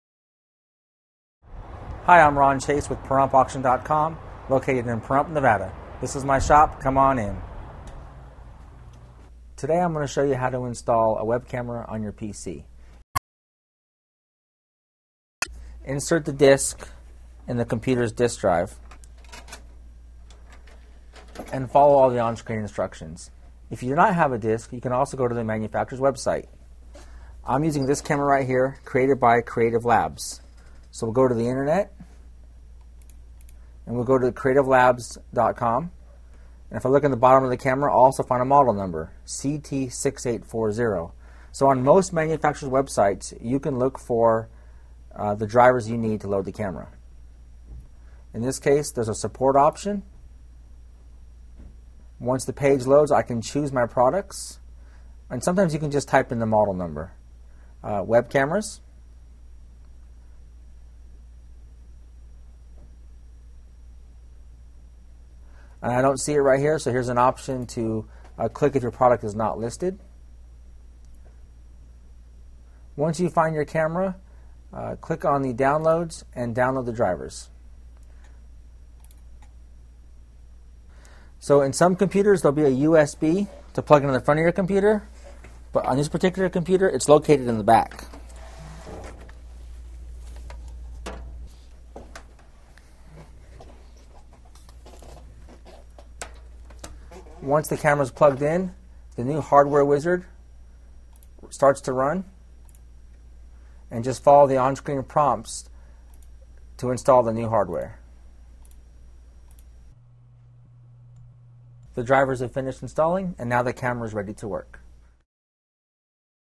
How to Connect a Webcam
How to Connect a Webcam. Part of the series: Computer Hardware Basics. A webcam can be set up and installed on a computer using the manufacturer's disc or following the instructions on the manufacturer's website to download the drivers needed to load the camera. Learn how to set up your webcam with the advice of a computer hardware professional in this free video on computer hardware basics.